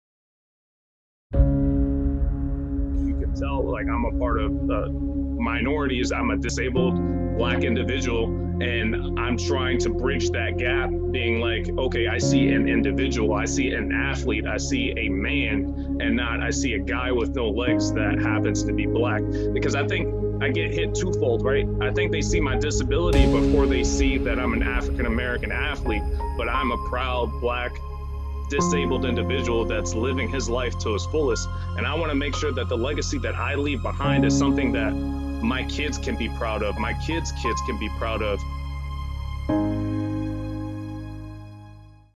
Citi: Trey Jenifer – Leaving a Legacy
Paralympic gold medalist Trevon “Trey” Jenifer discusses the impact he wants to leave as a proud, black athlete while living his life to his fullest.